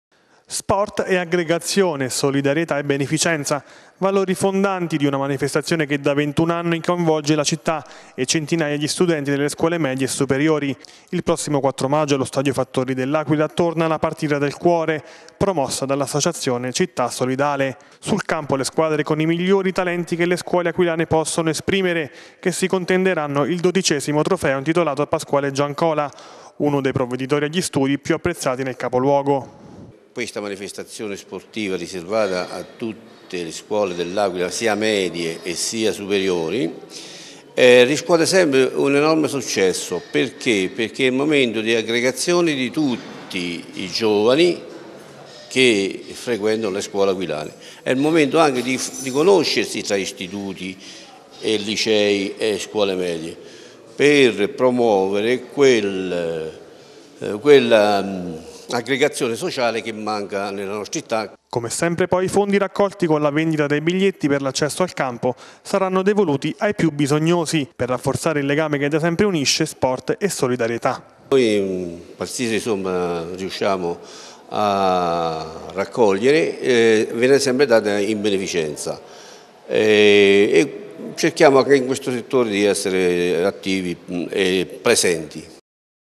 Sport e solidarietà: torna la partita del cuore
Centinaia di studenti delle scuole medie e superiori del capoluogo si affronteranno il prossimo 4 Maggio, allo Stadio Fattori, nel corso della partita del cuore; l'iniziativa coniuga sport e solidarietà